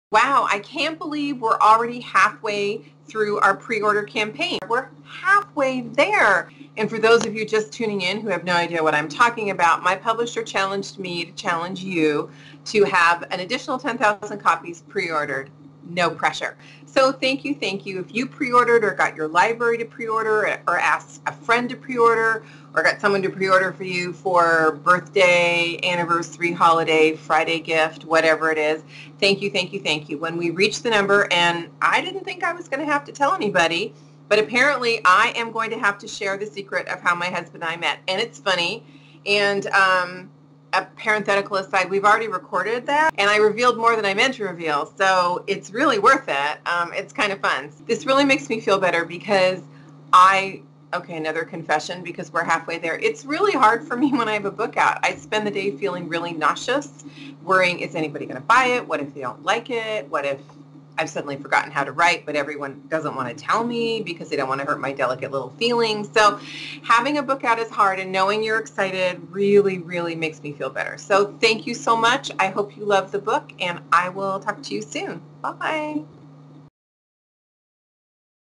Secrets of the Tulip Sisters Preorder Report
Susan Mallery's publisher challenged her readers to help get an additional 10,000 preorders of Secrets of the Tulip Sisters, Susan's 2017 summer beach book. In this video, Susan gives a progress report.